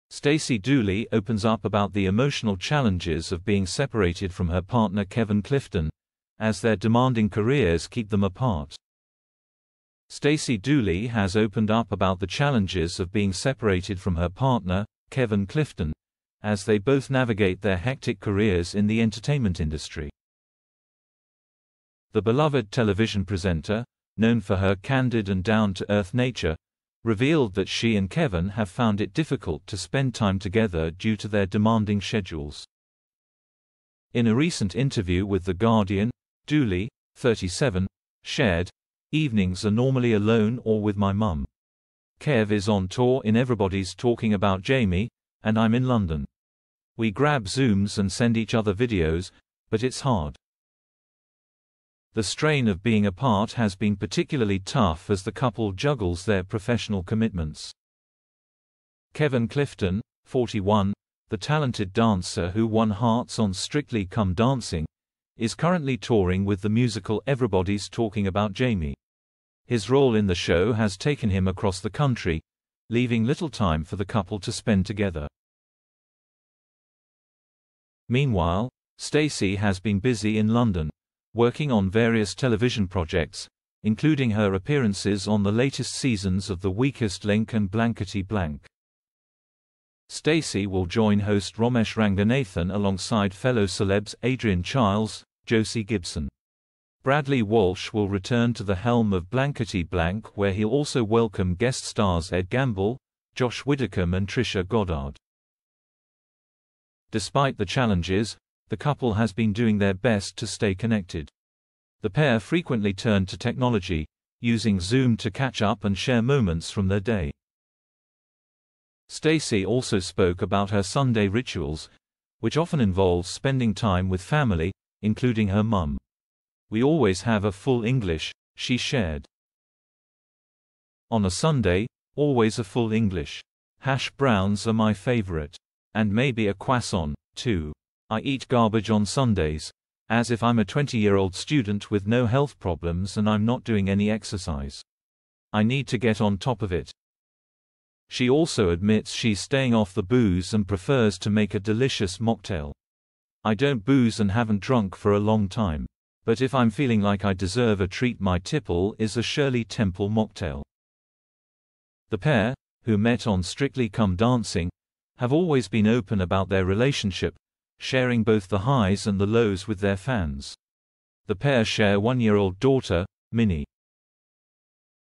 Strictly's Stacey Dooley admits ‘it’s hard’ as she spends time away from Kevin Clifton
Stacey Dooley opens up about the emotional challenges of being separated from her partner Kevin Clifton, as their demanding careers keep them apart.